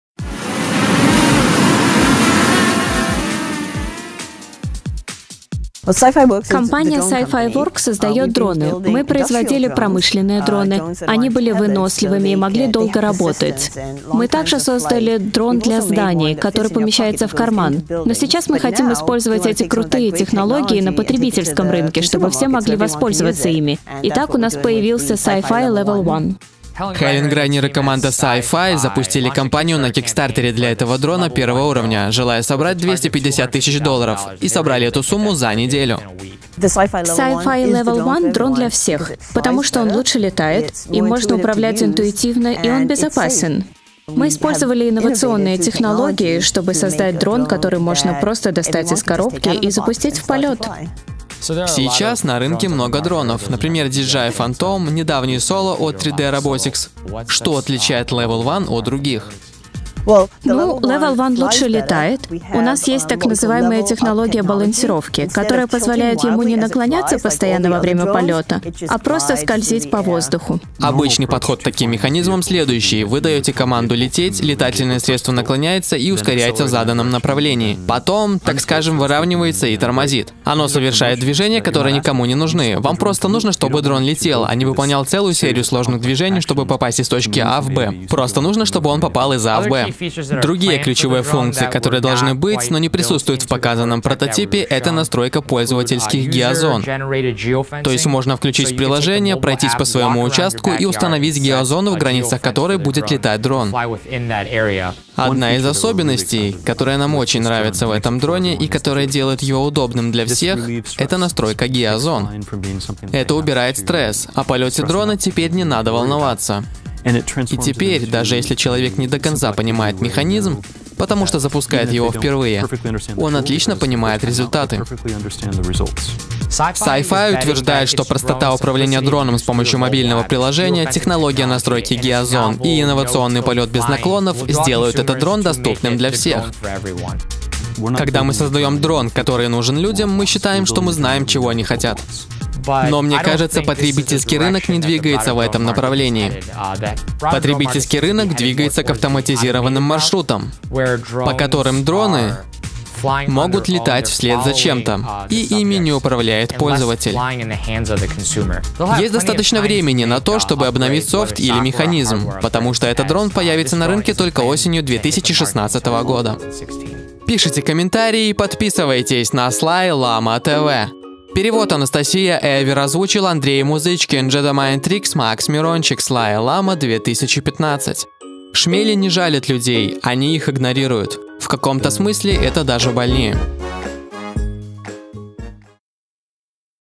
Forbes по-русски: CyPhy LVL 1 - дрон для всех
© Forbes // Перевод - SlyLama

CyPhy - это компания, разрабатывающая дроны на протяжении уже некоторых лет, но до этого фокусировавшаяся на промышленных или военных задачах. Их новая разработка - CyPhy Leve 1 - это еще одна попытка сделать дрон, понятный и удобный для всех. Что он может предложить и чем отличается от конкурентов, рассказывают главный инженер проекта и глава компании.